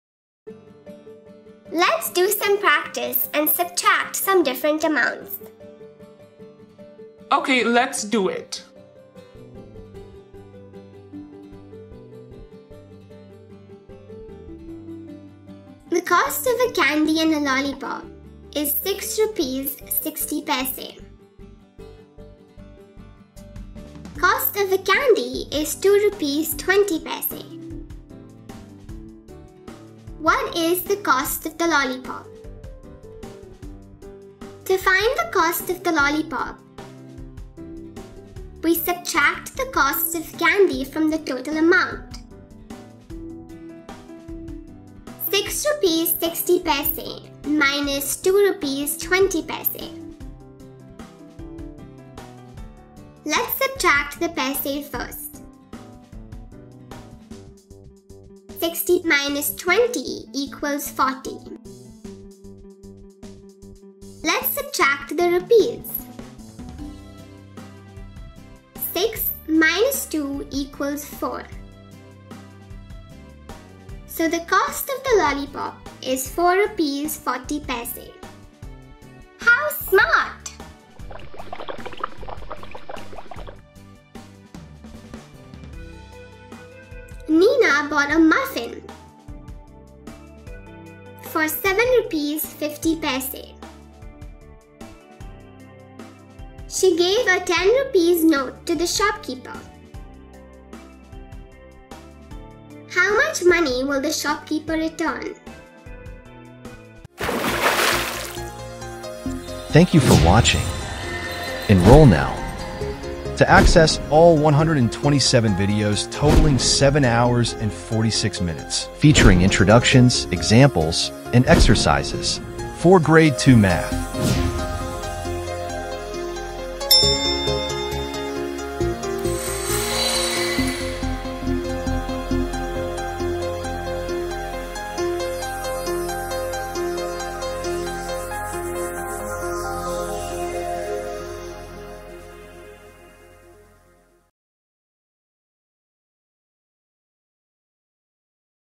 G2 - Module 18 - Exercise 6 - Statement problems - Subtraction | Appu Series | Grade 2 Math Learning
Can you solve these real-world subtraction problems? Appu and Tiger are here to guide you through statement problems where you’ll subtract amounts and strengthen your money management skills. Let’s have some fun with numbers!